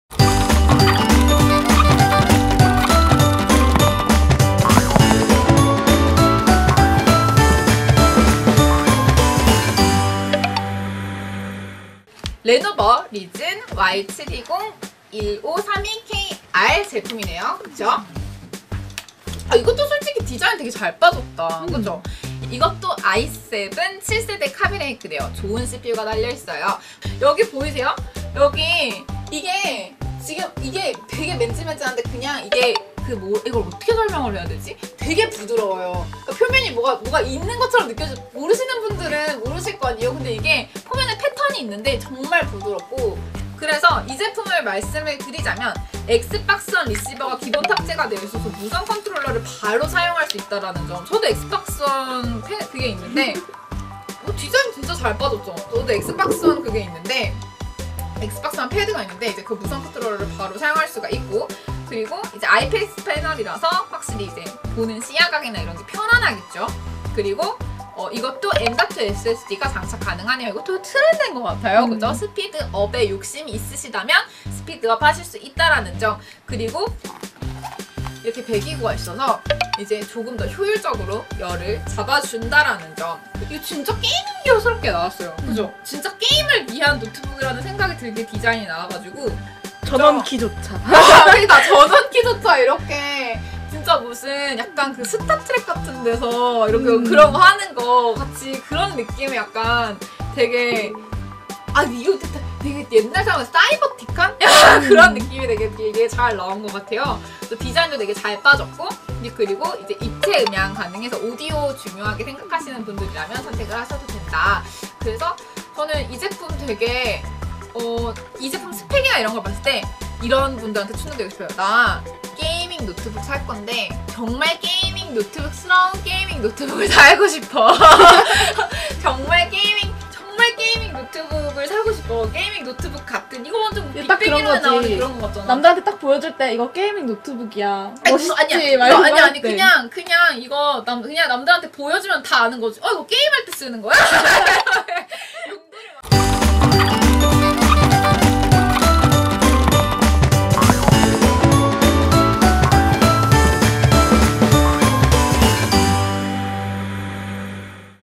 레노버 LEGION Y720 15 32KR 1TB 2017년 4월 다나와 표준노트북
맹여왕이 추천하는 4월의 표준노트북! 레노버 LEGION Y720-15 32KR (1TB)